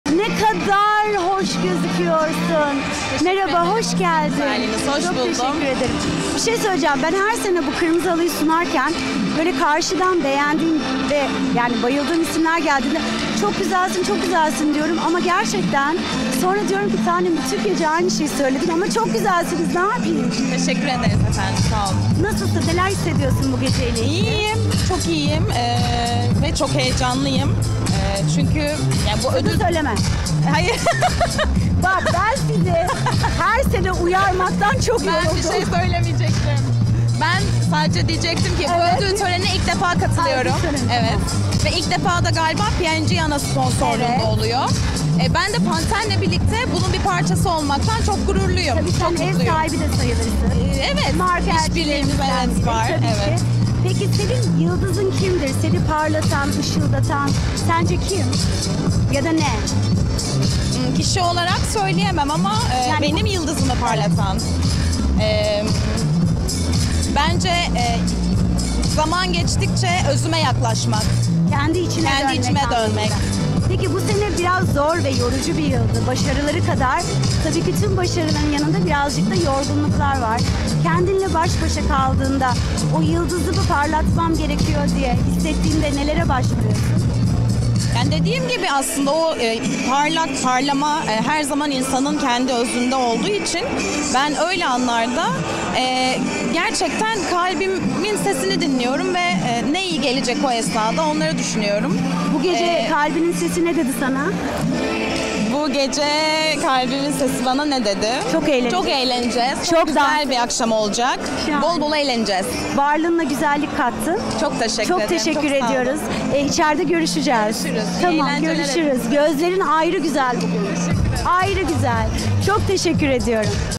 ELLEStyleAwards2024 | Sıla Türkoğlu
Tanem Sivar, ELLE Style Awards 2024'te canlı yayınlanan kırmızı halı röportajında Sıla Türkoğlu ile keyifli bir sohbet gerçekleştirdi.